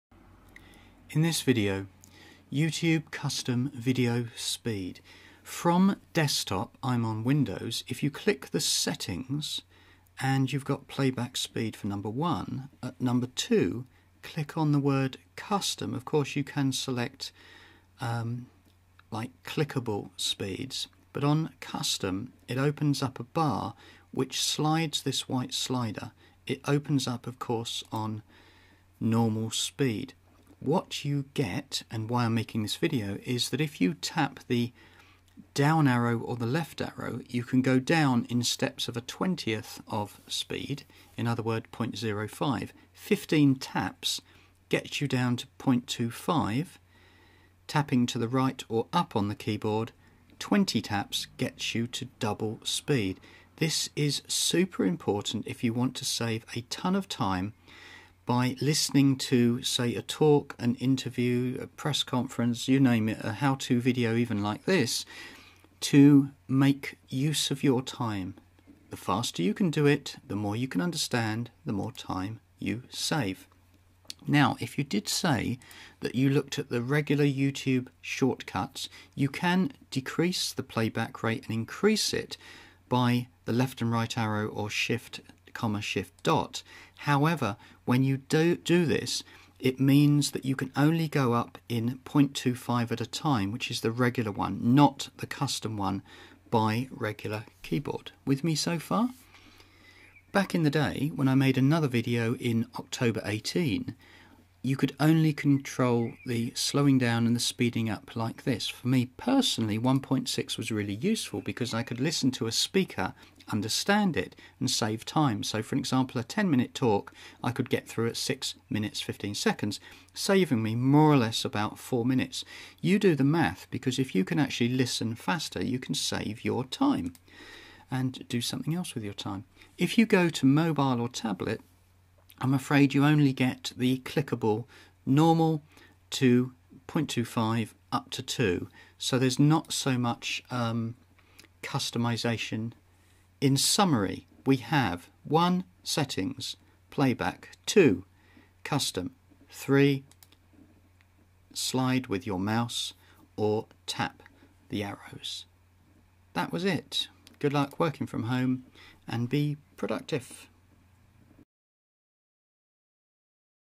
YouTube Custom Video Speed for Work from Home Productivity
On desktop we now have a Custom Playback Speed slider that allows the keyboard arrows keys when tapped to change playback speed at a rate of 0.05 per tap. This is very useful if you wish, and need, to be super pruductive to make sense of a video at a faster rate.

My optimal speed is about x1.6 meaning a 10 minute video will take me 6 mins to watch thus saving 4 mins. Therefore an hour long meeting or lecture could take just 40 mins, saving you the 20 minutes for something else .. \o/ ..

It seems on mobile/tablet you can only access the 0.25 spped ontervals, so for me, i'm stuck with x1.5 insted of the x1.6 i would like. With this method of using the laft/right up/down arrows, we get:
-- 15 increments down ie 0.25, 0.30, 0.35, 0.40, 0.45 … 1.00 then
-- 20 increments down 1.00, 1.05, 1.10, 1.15, 1.20 … 2.00 or
-- control by a mouse drag where keyboard arrows all 1/20th of speed per tap.